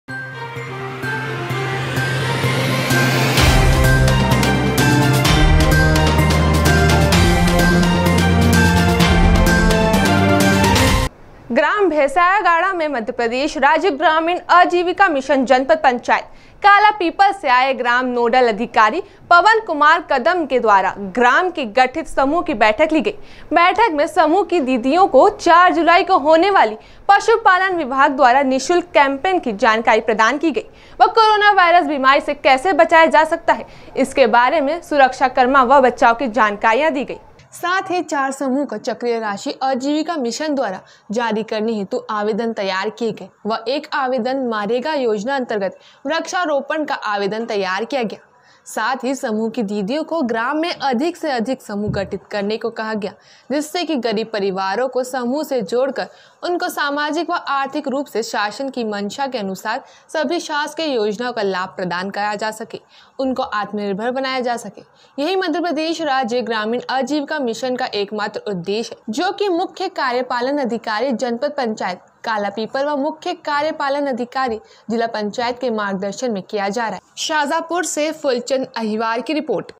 शाजापुर-ग्राम नोडल अधिकारी द्वारा ग्राम में गठित समूहो की बैठक ली गई
साथ ही 04 समूहो को चक्रीय राशि आजीविका मिशन द्वारा जारी करने हेतु आवेदन तैयार किये गए व 01 आवेदन मनरेगा योजनान्तर्ग वृक्षारोपण का आवेदन तैयार किया गया , साथ ही समूह  कि दीदियों को ग्राम में अधिक से अधिक समूह गठित करने को कहा गया जिससे कि गरीब परिवारों को समूह से जोड़ कर उनको सामाजिक व आर्थिक रूप से शासन की मंशा के  आनुसार सभी शाशकीय योजनाओं का लाभ प्रदान किया जा सके और उनको आत्मनिर्भर बनाया जा सके. यही मध्यप्रदेश राज्य ग्रामीण आजीविका मिशन का एक मात्र उद्देश्य है जो की  मुख्य कार्य पालन अधिकारी जनपद पंचायत कालापीपल व मुख्य कार्य पालन अधिकारी जिला पंचायत के मार्गदर्शन में किया जा रहा है